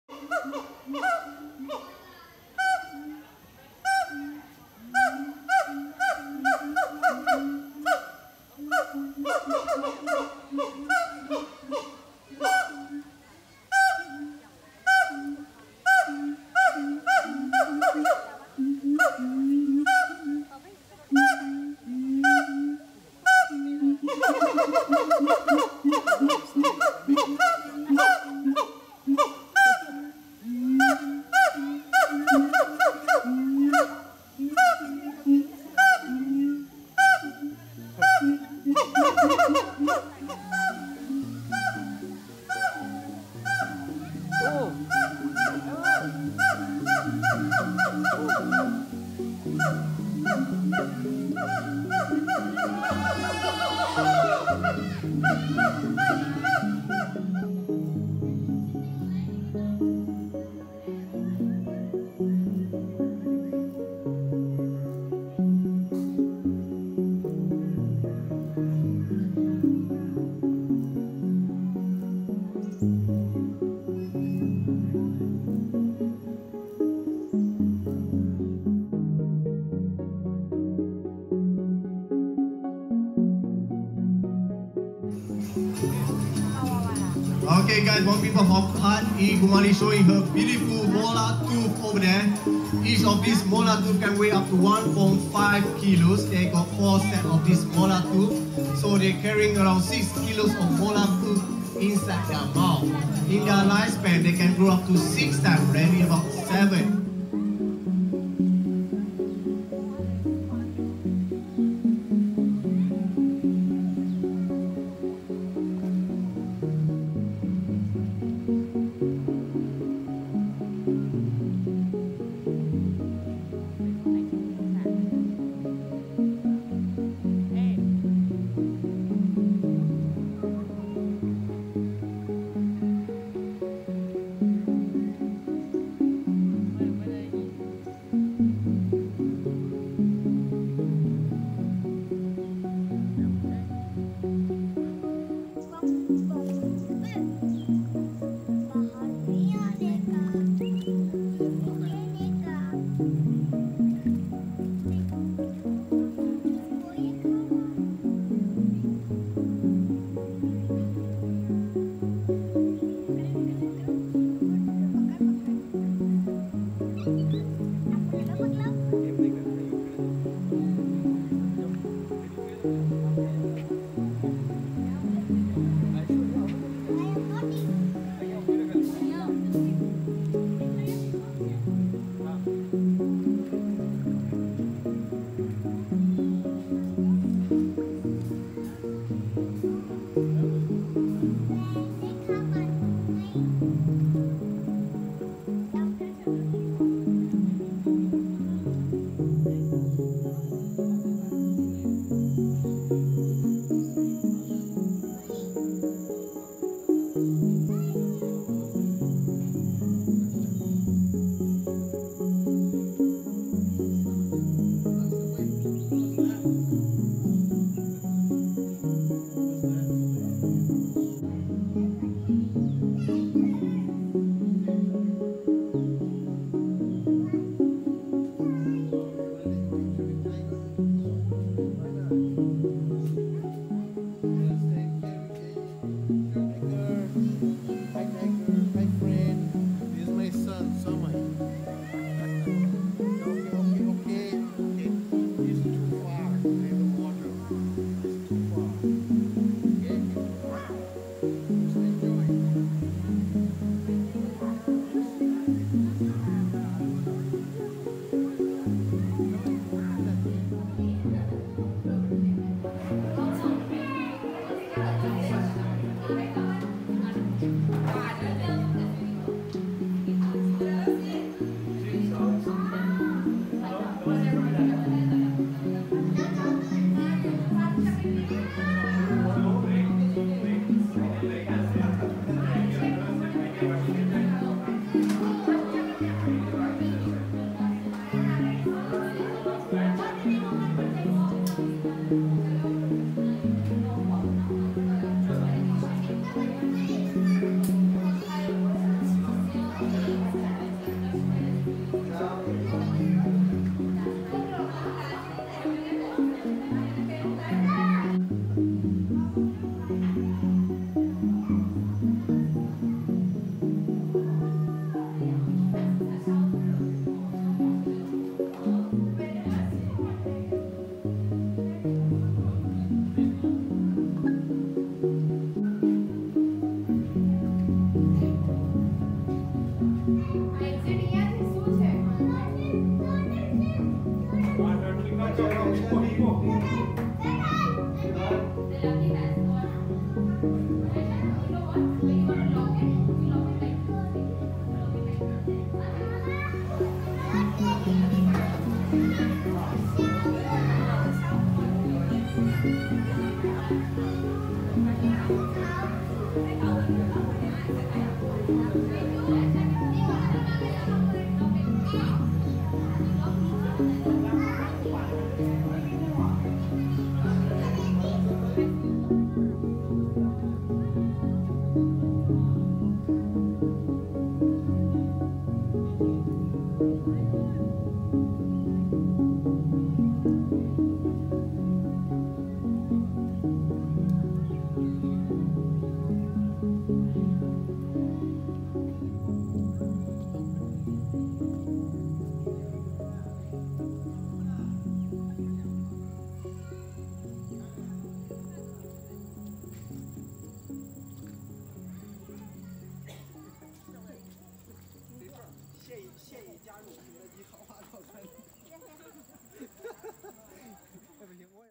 What to look for in Singapore Zoo (Xem gì ở sở thú Singapore) | Place 2 Visit
You can find thousands of animals and plants in Singapore Zoo. But these are some highlights of our visit that you might want to look for if you happen to come to the zoo.
We actually saw the lions but could not video them clearly 😉

★ Source: namnews365